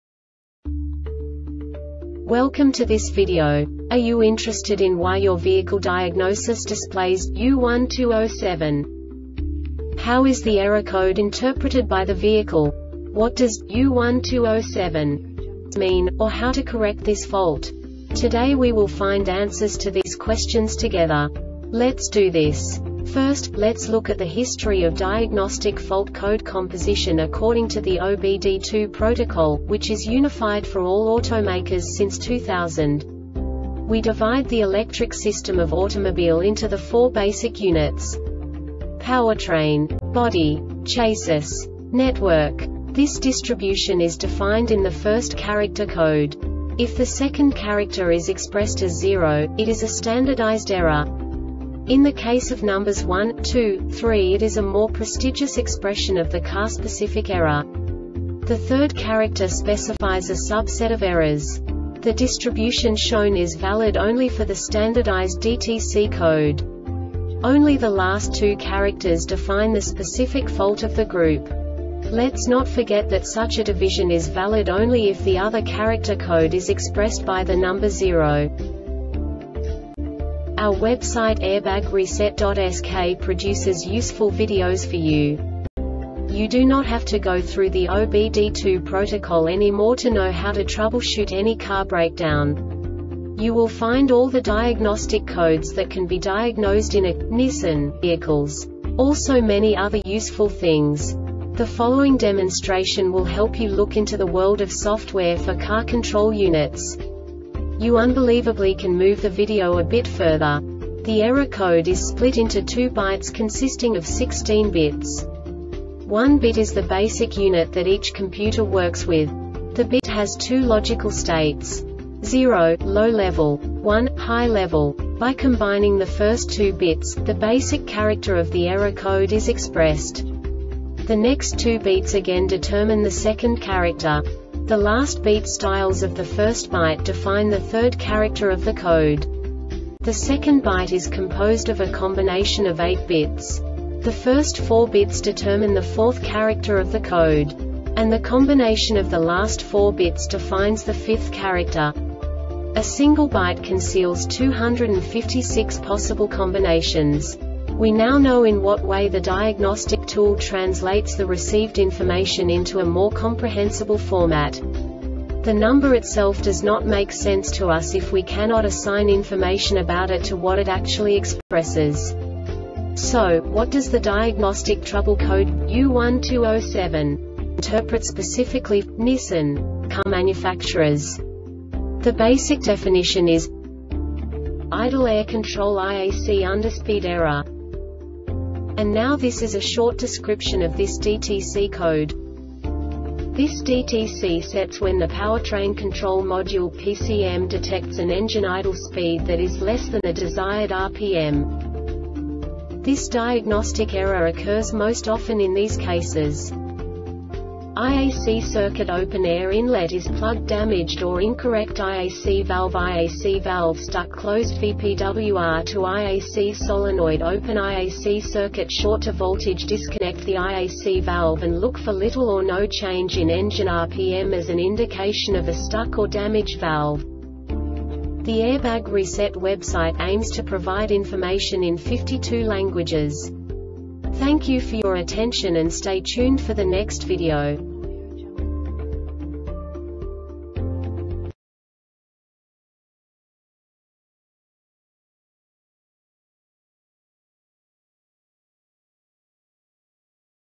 DTC Nissan U1207 Short Explanation
Contents:
0:21 Basic DTC analysis according to OBD2 protocol standard.
1:48 Insight into programming
2:58 A diagnostically understandable description of the Diagnostic Trouble Code
3:05 Basic error definition

Make:
Nissan
Code:
U1207
Definition:
Idle Air Control (IAC) Underspeed Error
Description:
This DTC sets when the powertrain control module (PCM) detects an engine idle speed that is less than the desired RPM.
Cause:
IAC circuit openAir inlet is pluggedDamaged or incorrect IAC valveIAC valve stuck closedVPWR to IAC solenoid openIAC circuit short to voltage
Diagnosis:
Disconnect the IAC valve and look for little or no change in engine RPM as an indication of a stuck or damaged valve.